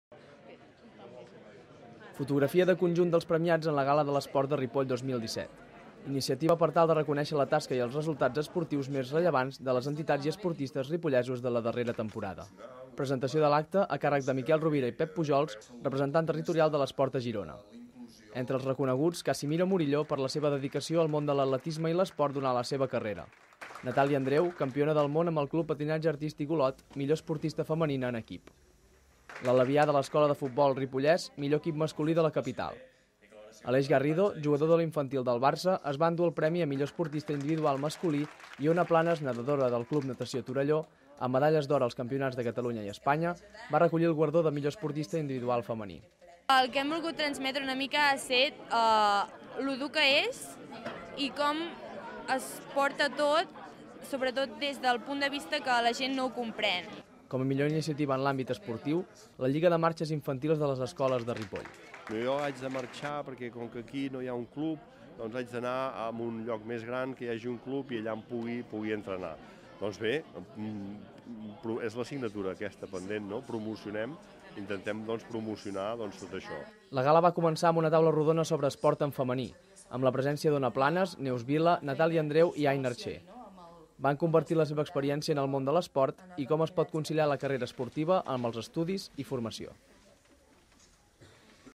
Ripoll reconeix Ona Planas com a millor esportista femenina
Fotografia de conjunt dels premiats en la Gala de l'Esport de Ripoll 2017. Iniciativa per tal de reconèixer la tasca i els resultats esportius més rellevants de les entitats i esportistes ripollesos de la darrera temporada. Presentació de l'acte, a càrrec de Miquel Rovira i Pep Pujols, representant territorial de l'esport a Girona. Entre els reconeguts, Casimiro Murillo per la seva dedicació al món de l'atletisme i l'esport durant la seva carrera. Natàlia Andreu, campiona del món amb el Club Patinatge Artístic Olot, millor esportista femenina en equip.